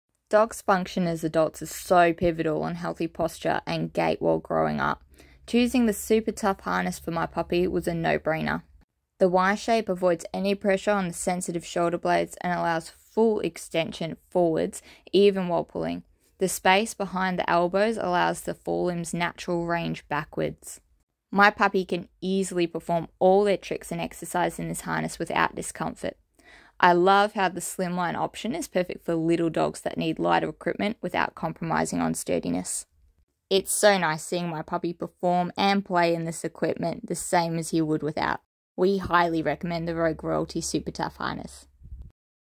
Want the perfect lightweight harness for an active dog? Slim Fit SupaTuff® dog harness overview.
A customer breakdown of our slim fit SupaTuff® dog harness which is the perfect combination of ergonomics, strength, simplicity and comfort that is excellent for puppies and dogs of all ages and sizes.
This dog harness is highly recommended for:
1. pups and dogs undergoing training that requires a secure, strong harness for both dog and handler safety.
2. Use in all weather conditions as it's rot and rust resistant and can endure harsh rugged environments
3. Trainers, handlers or owners who want no plastic fittings only stainless steel.